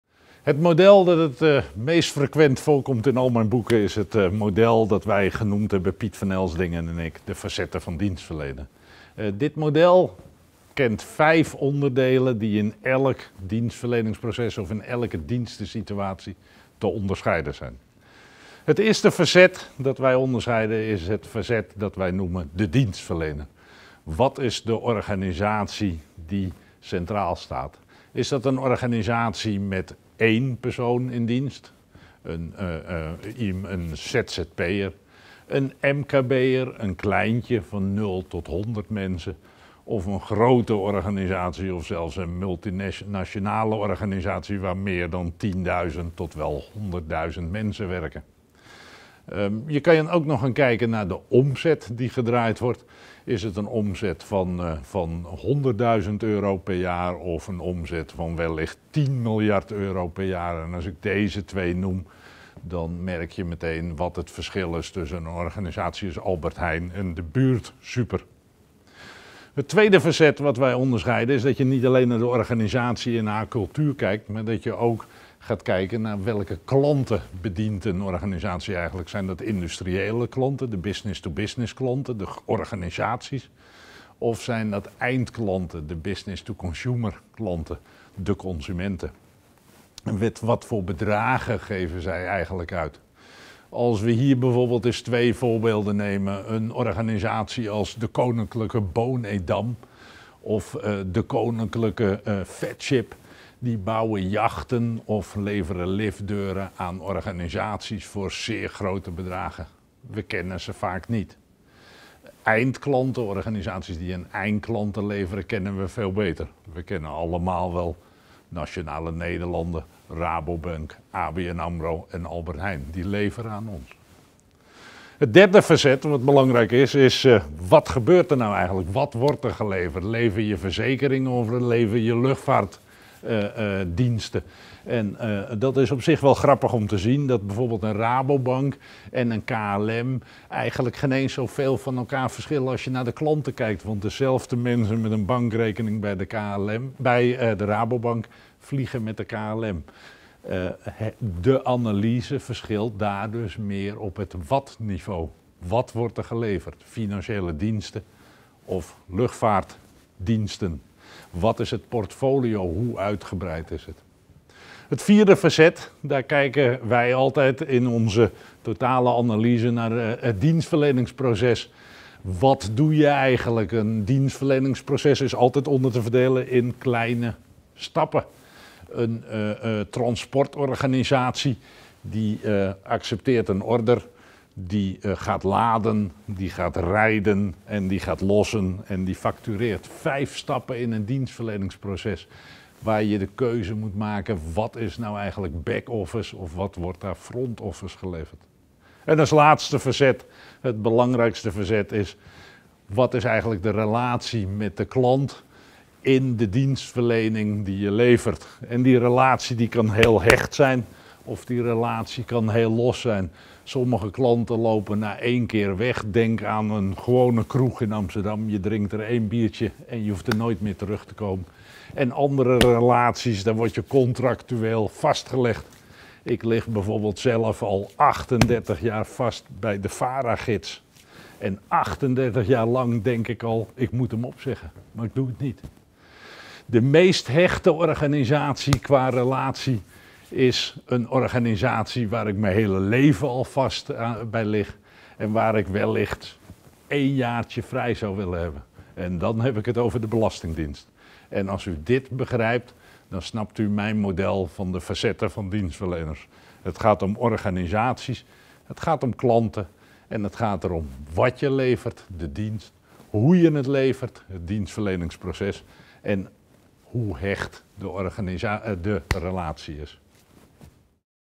De facetten van Dienstverlenen (met model)
Wouter de Vries van bestseller Blauwe bananen verteld de basisprincipes van dienstverlening.